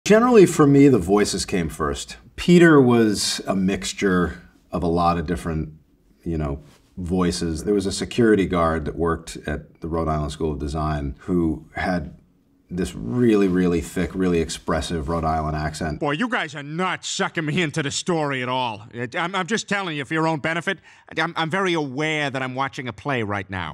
How Seth MacFarlane came up with Peter Griffin's voice
Seth MacFarlane explains how Peter Griffin's voice came to life for Family Guy.